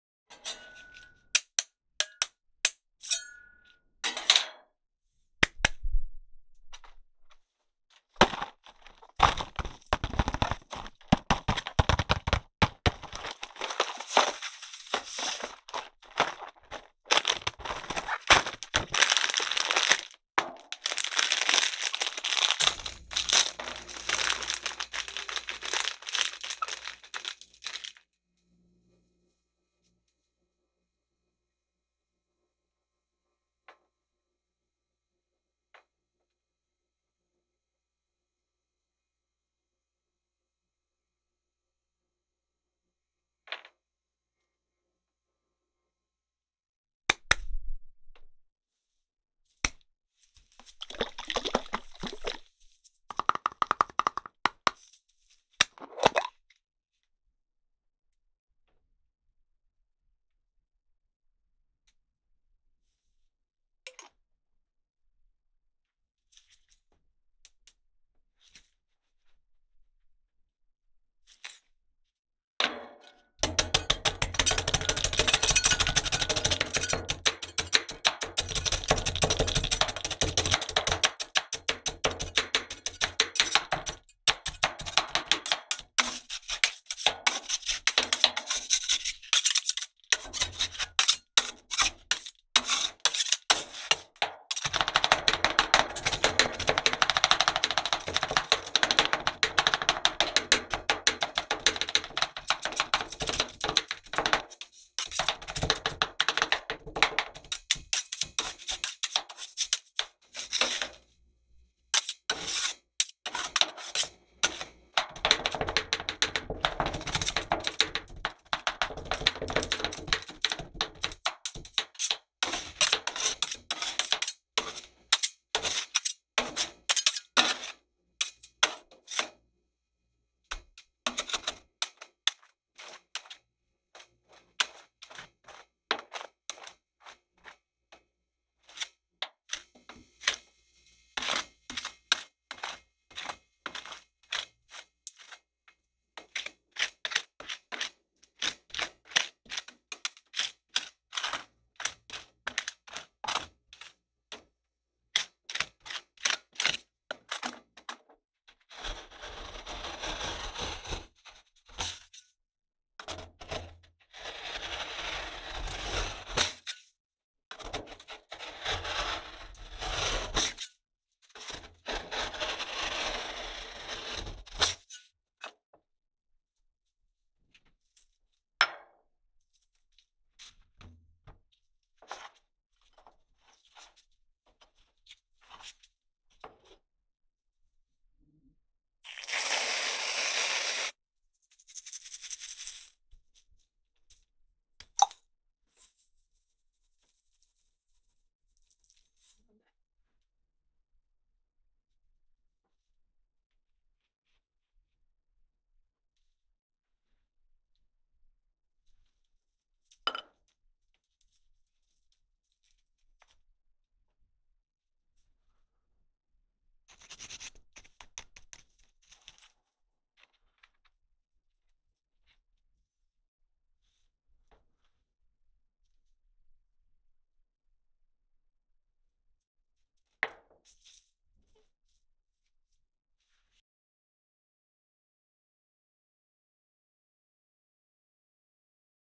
칸초로 아이스크림 만들기 / ASMR / Ice cream made with Cancho
"안녕하세요~
오늘은 Cancho 아이스크림을 만드는 영상을 가져왔습니다

다음 영상을 기다려주세요~

hello~
Today I brought a video of making cancho ice cream

Please enjoy the video today
Please wait for the next video ~"